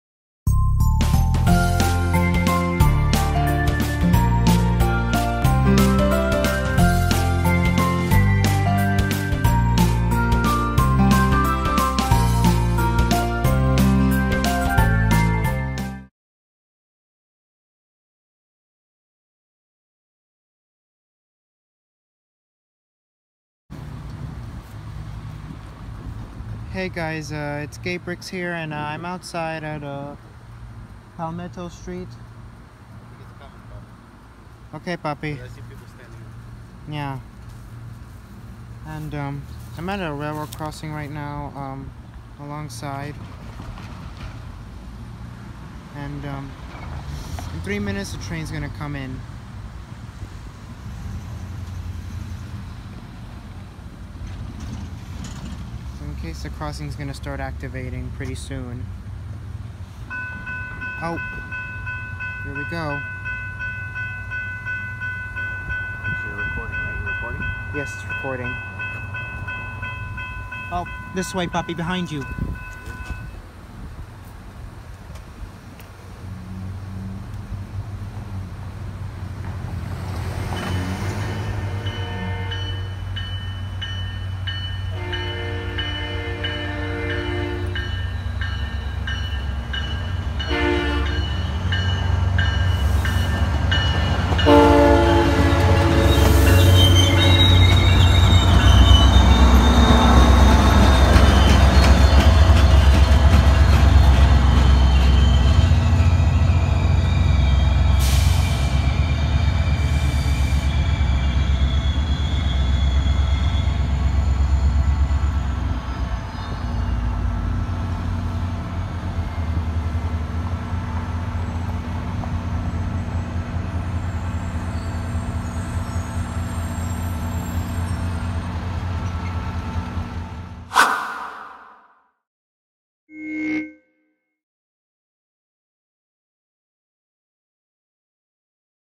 SunRail Train In Lake Mary, Palmettos St FL 1080p HD 60fps
I decided to go out and film a train passing through a railroad crossing.